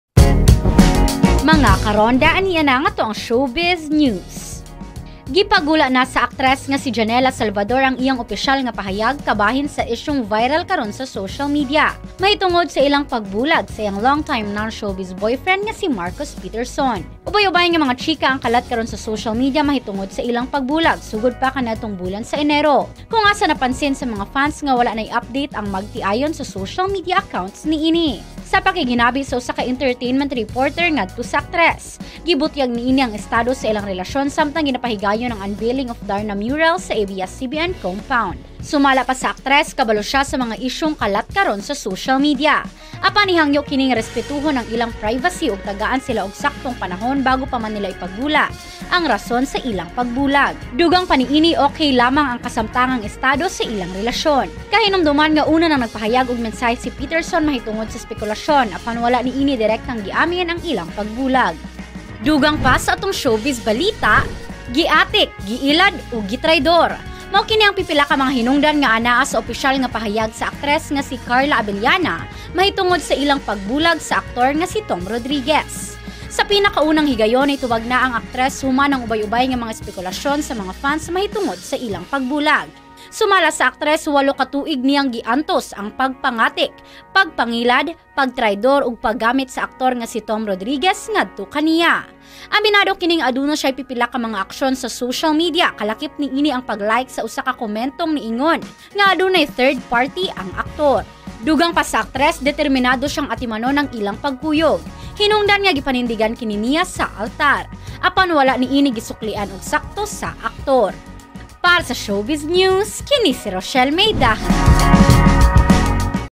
SHOWBIZ NEWS - 06/18/22
Showbiz 01: JANELLA SALVADOR, NIHANGYO SA MGA FANS NGA RESPETUHON ANG ILANG PRIVACY KABAHIN SA ISYUNG PAGBULAG NILA NI MARKUS PATERSON

Showbiz 02: CARLA ABELLANA, GIPAGULA NA ANG IYANG OPISYAL NGA PAHAYAG KABAHIN SAILANG PAGBULAG SA AKTOR NGA SI TOM RODRIGUEZ

Ronda Report Showbiz News:
ROCHELLE MAE DAHAN
Ronda Patrol 26